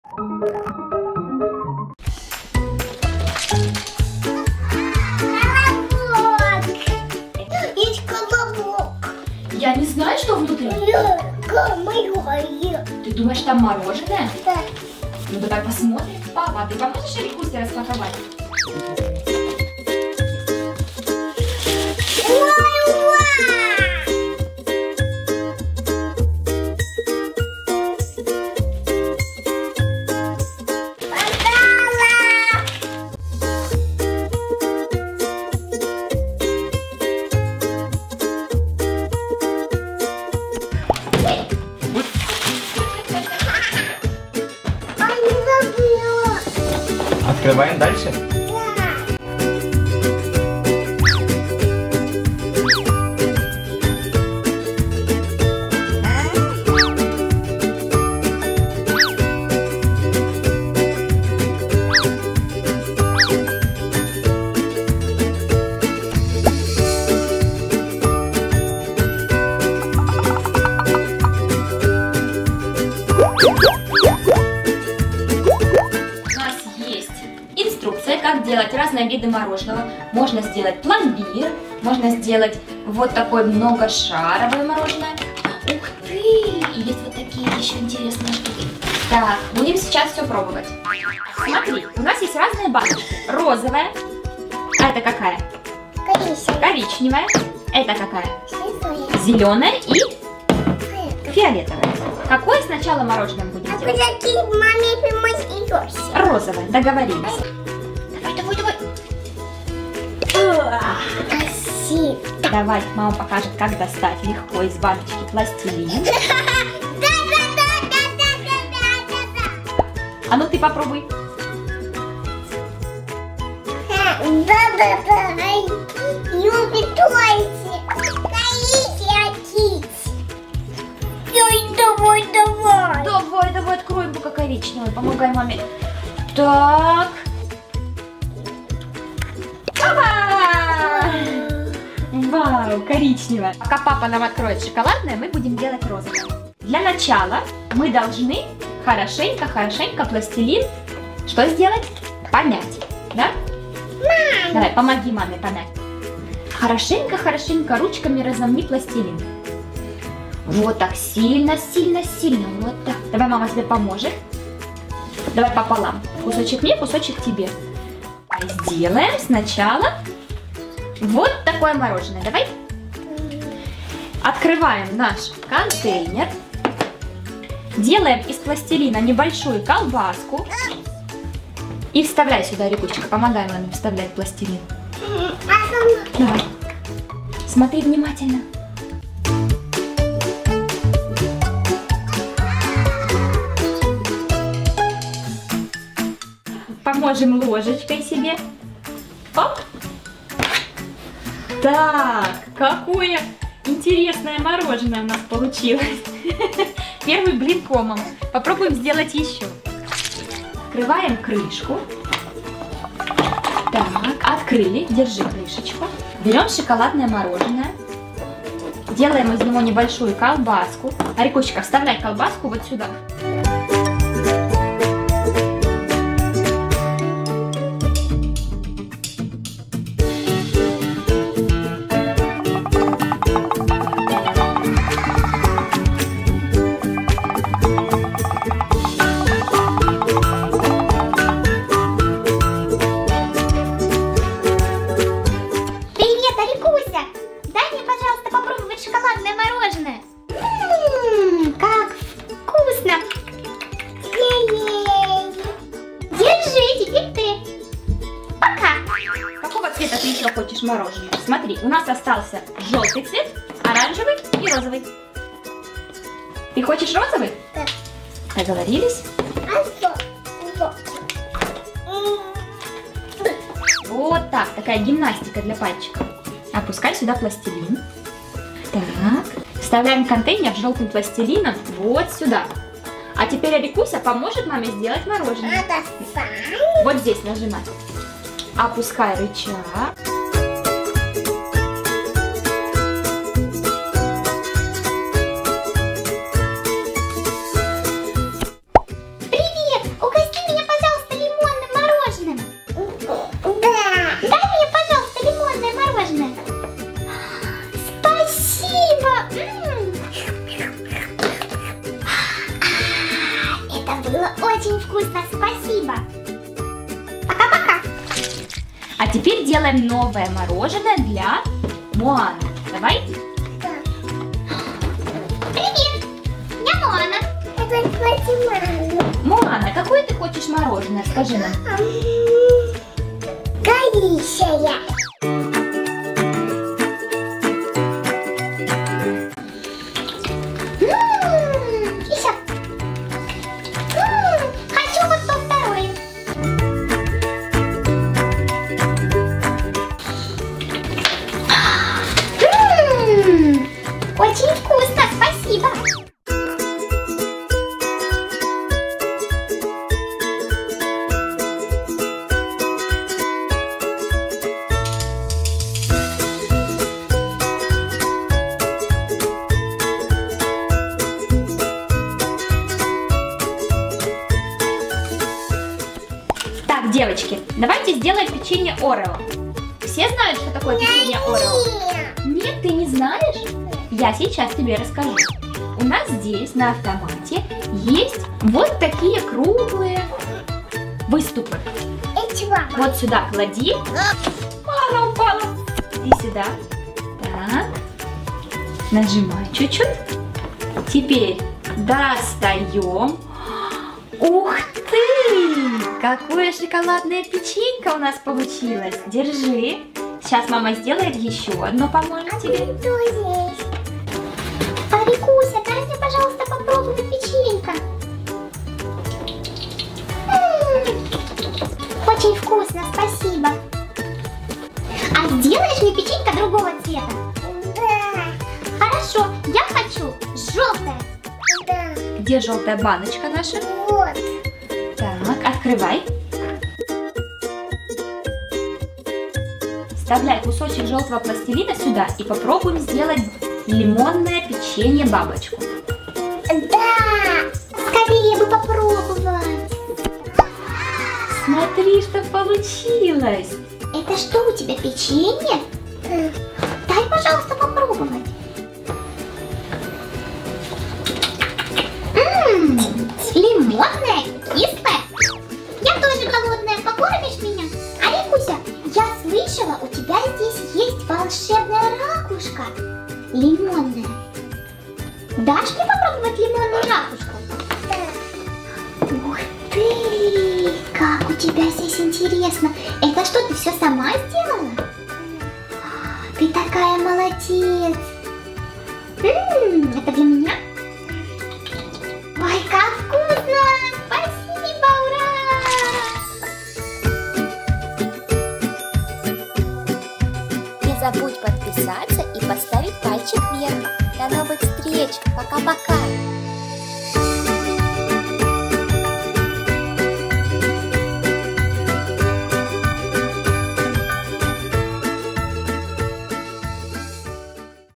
Принцесса Ариэль, Моана и Ариэла готовят мороженое | пластилин Play-Doh Kitchen creations
Ариэла распаковывает большой набор для творчества от Play-Doh Kitchen creations, лепит из пластилина, готовит мороженое и угощает им своих кукол - принцессу Ариэль и Моану.